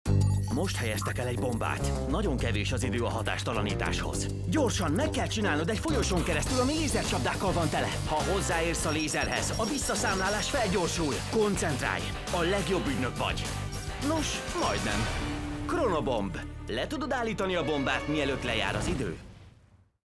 Chrono Bomb - Gyerekjatekok.hu
Chrono Bomb - Mentsd meg a világot! ügyességi társasjáték
Ha elég ügyes és gyors vagy, megmentheted a világot! A Chrono Bomb egy izgalmas játék, amit csapatban, de akár egyedül is játszhatsz. Telepítsd könnyedén a védelmi zónát, válaszd ki a kívánt játékidőt és nyomd meg a START gombot! A játék elkezdődött! Mássz be a zónába anélkül, hogy hozzáérnél a zsinórhoz! Ha véletlenül megérinted a zsinórt, a visszaszámlálás felgyorsul. Amint odaértél a bombához, hatástalanítsd, mielőtt túl késő lenne!